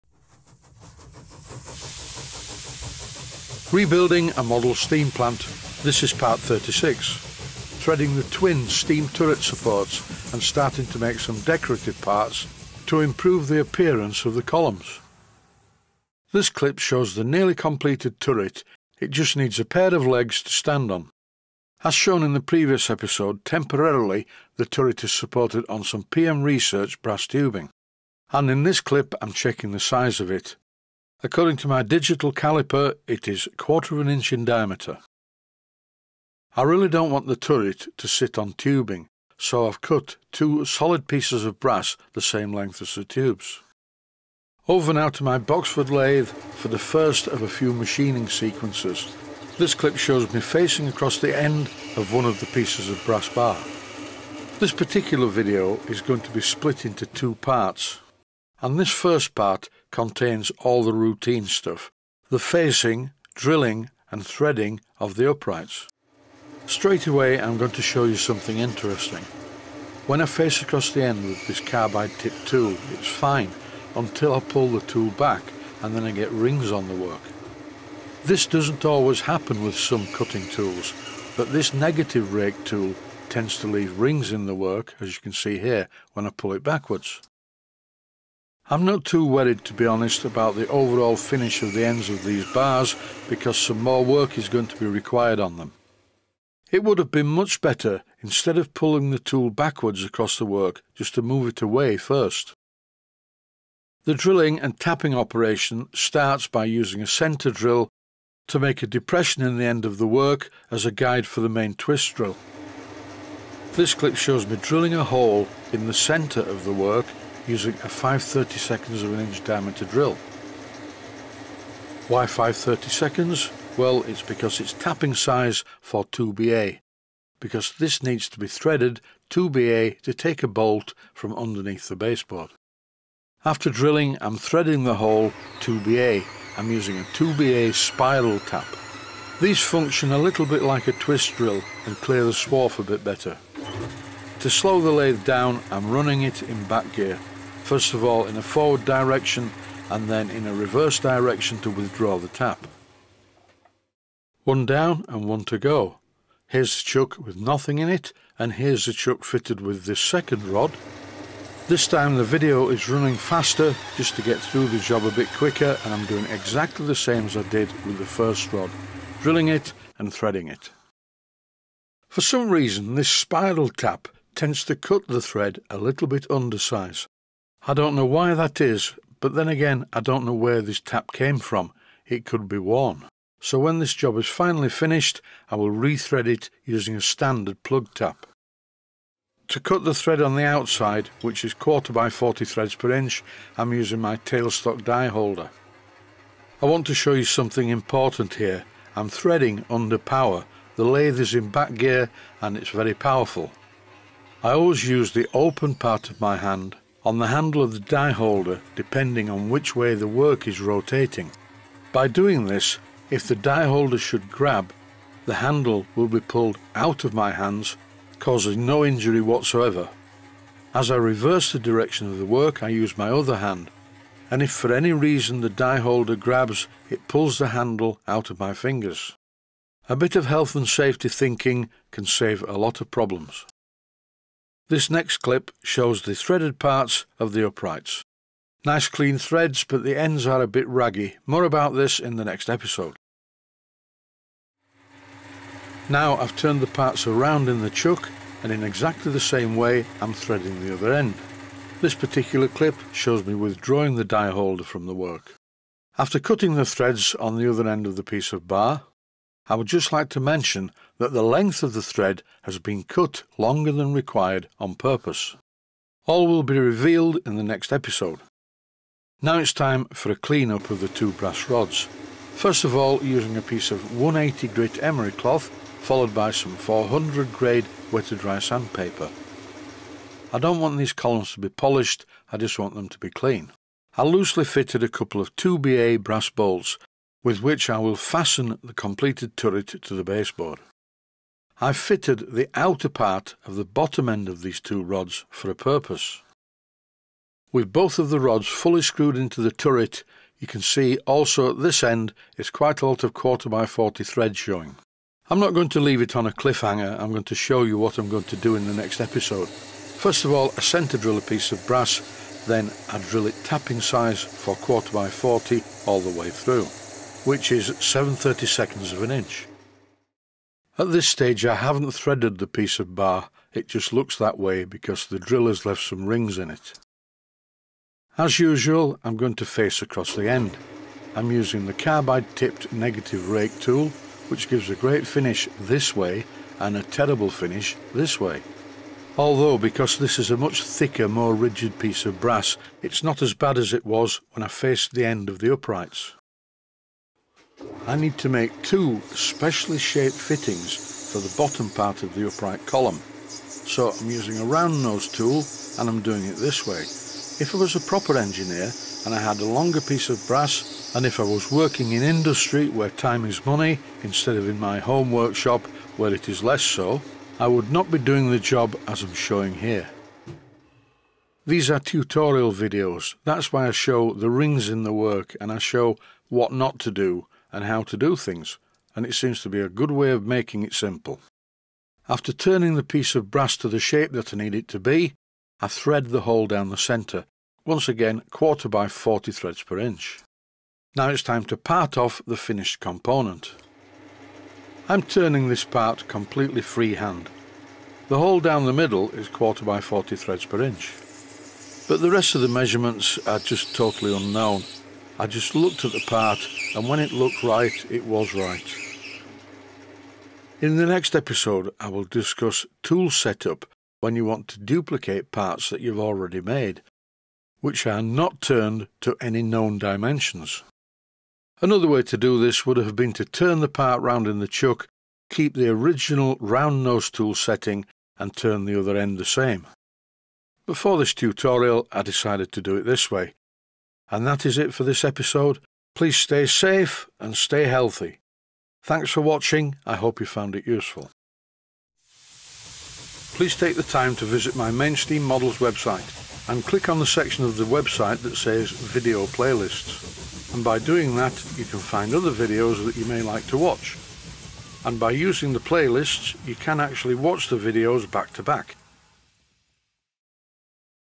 REBUILDING A MODEL STEAM PLANT - PART #36
Rebuilding A Model Steam Plant - Part #36. Threading the twin Steam Turret supports and starting to make some decorative parts to improve the appearance of the columns.